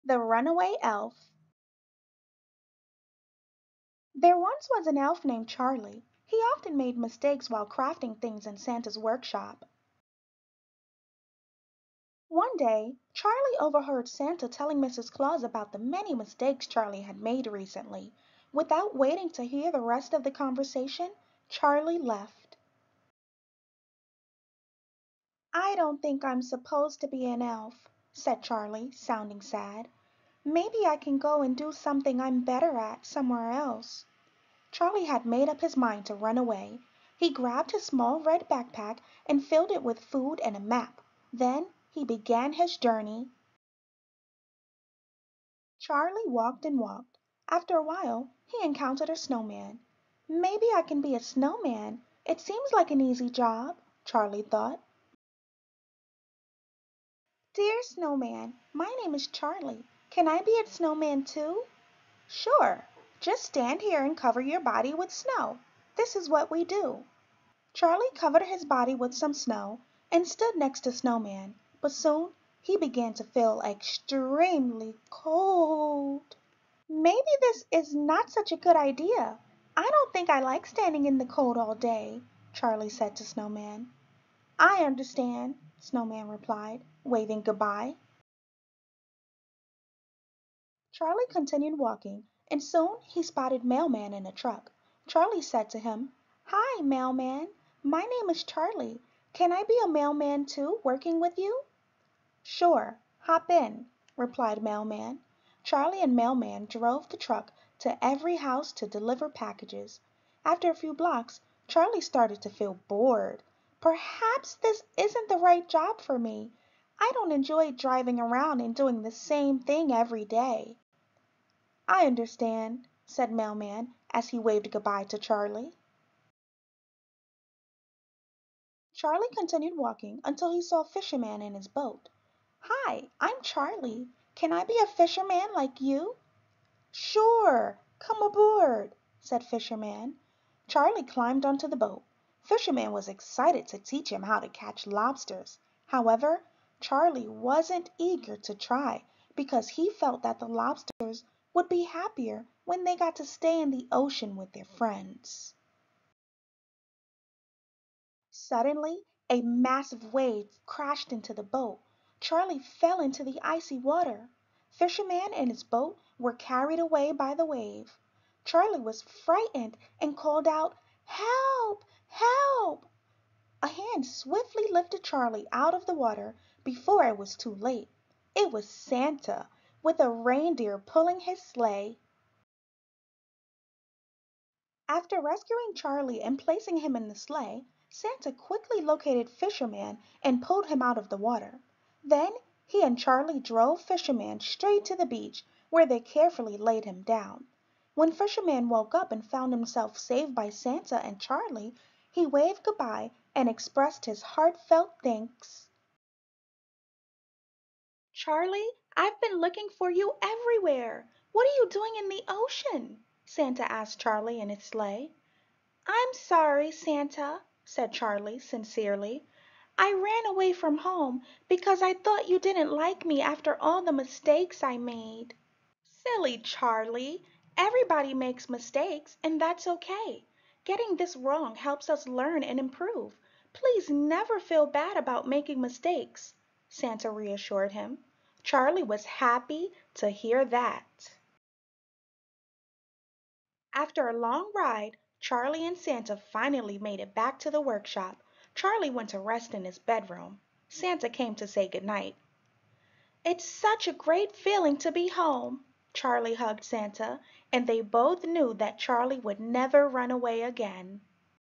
"The Runaway Elf" - Will Santa's Elf Return? (Christmas Story for Ages 3-7)
One of Santa's elves runs away and learns a valuable lesson by the end of the day.